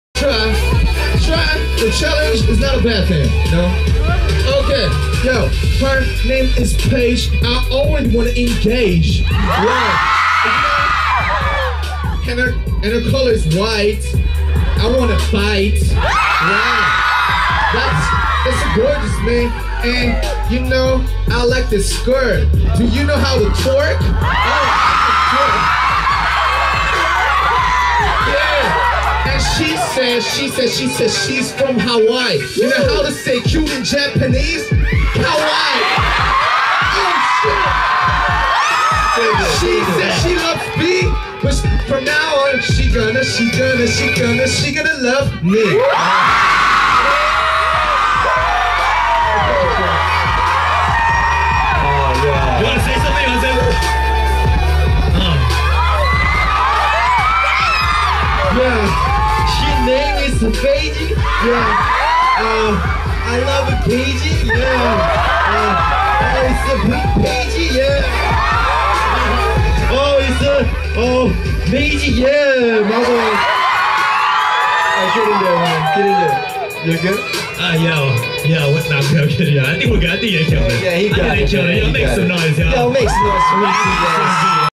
[KCON 2014 FANCAM] BTS DFLA - Rap Mon Freestyle Rap About Fan Diana TvSanders
"I like this skirt, do you know how to twerk?"
OMGGGG.